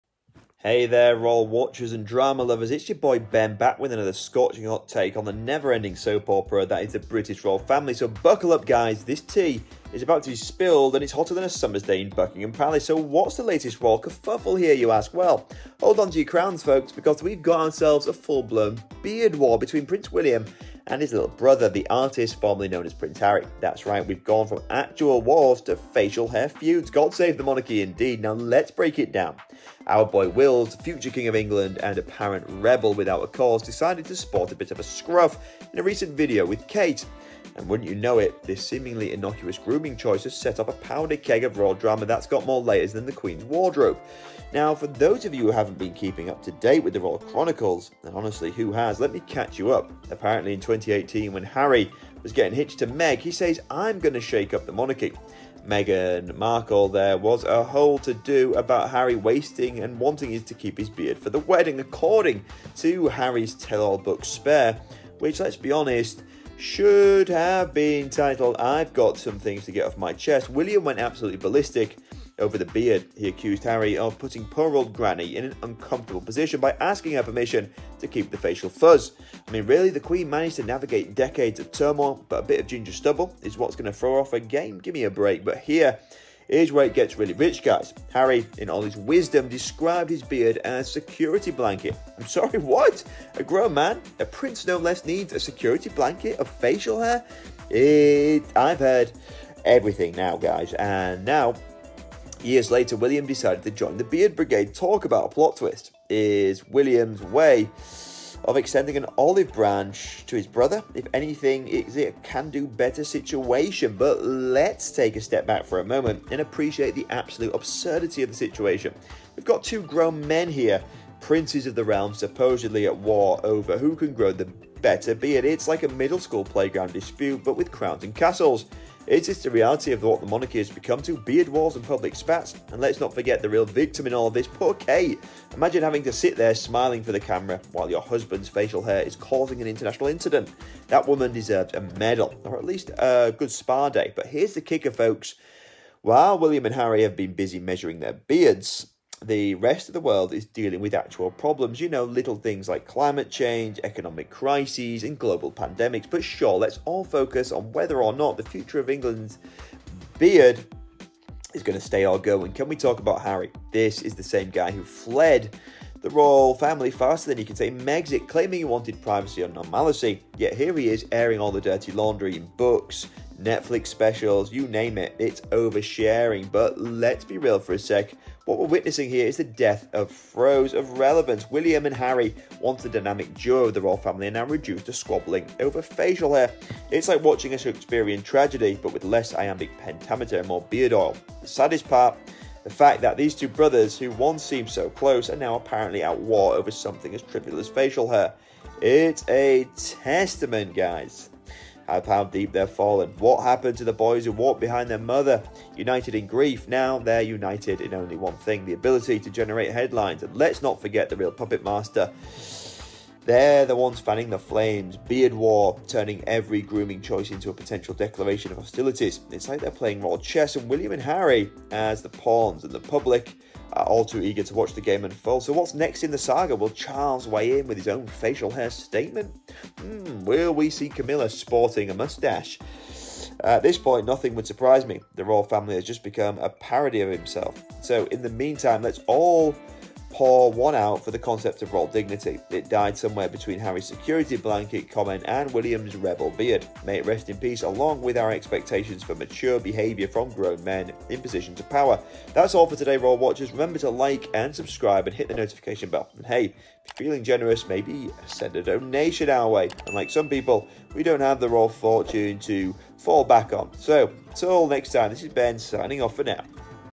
THIS IS THE END!’ Furious Prince William DECLARES WAR On Harry After Latest Move!
Forget the crown—it's all about the beards now! Prince William and Prince Harry are at it again, this time embroiled in a whisker war that's got the monarchy buzzing. William's latest scruffy appearance has sparked a royal uproar, with echoes of Harry's 2018 beard debacle during his wedding to Meghan Markle. Remember when Harry called his beard a "security blanket"? Well, now it seems William’s ready to join the beard brigade, reigniting old tensions.

But this isn’t just about grooming—it’s a metaphorical battlefield where old grudges are rehashed and royal egos clash. Is William's new look a peace offering or a challenge? As the world grapples with real issues, the Windsor brothers are busy measuring facial fuzz. Who knew royal drama could be so... hairy?